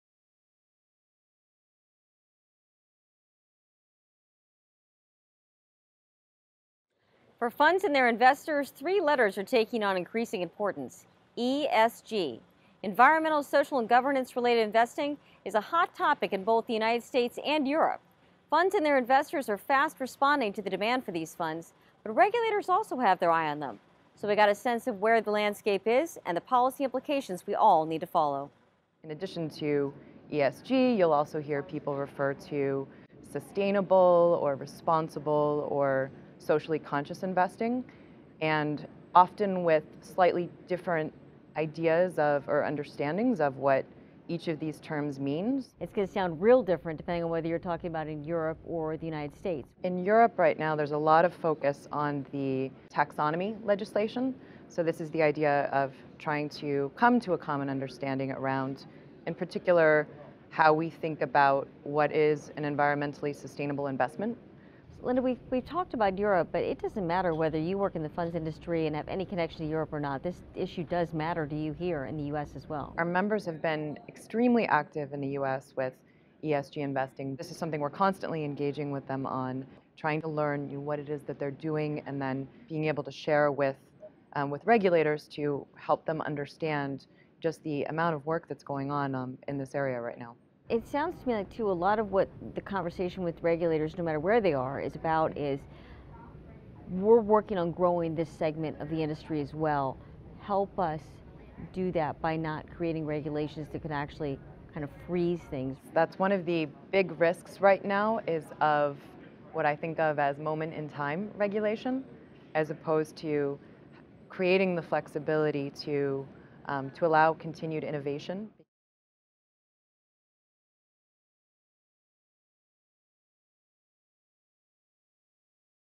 Focus on Funds: Global Interest in Sustainably Focused Funds Is Growing
In Europe and the United States, regulatory and investor interest is growing in funds that meet certain “ESG”—environmental, social, and governance—criteria. In the June 13, 2019, edition of Focus on Funds, ICI Global Associate Chief Counsel Linda French discusses the latest developments.

Stephanie Ortbals-Tibbs, ICI director of media relations: For funds and their investors, three letters are taking on increasing importance: ESG. Environmental, social, and governance-related investing is a hot topic in both the United States and Europe. Funds and their investors are fast responding to the demand for these funds, but regulators also have their eye on them. So I got a sense of where the landscape is and the policy implications we all need to follow.

Linda French, ICI Global associate chief counsel, securities regulation: In addition to ESG, you’ll also hear people refer to sustainable or responsible or socially conscious investing, and often with slightly different ideas of—or understandings of—what each of these terms means.

Ortbals-Tibbs: It’s going to sound real different depending on whether you’re talking about Europe or the United States.

French: In Europe right now, there’s a lot of focus on the taxonomy legislation. So this is the idea of trying to come to a common understanding around, in particular, how we think about what is an environmentally sustainable investment.

Ortbals-Tibbs: Linda, we’ve talked about Europe, but it doesn’t matter whether you work in the funds industry and have any connection to Europe or not. This issue does matter to you here in the US as well.

French: Our members have been extremely active in the US with ESG investing. This is something we’re constantly engaging with them on, trying to learn what it is that they’re doing, and then being able to share with regulators to help them understand the amount of work that’s going on in this area right now.

Ortbals-Tibbs: It sounds to me like too a lot of what the conversation with regulators—no matter where they are—is about: “We’re working on growing this segment of the industry as well. Help us do that by not creating regulations that can actually kind of freeze things.”

French: That’s one of the big risks right now—what I think of as “moment-in-time” regulations, as opposed to creating the flexibility to allow continued innovation.